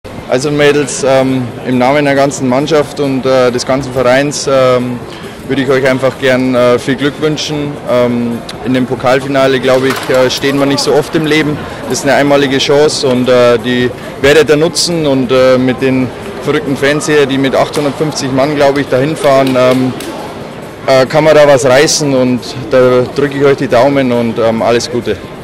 Patrick Wiegers Grußbotschaft
Grußbotschaft von Patrick Wiegers (SG Dynamo Dresden) zum Pokalfinale des DSC Volleyball.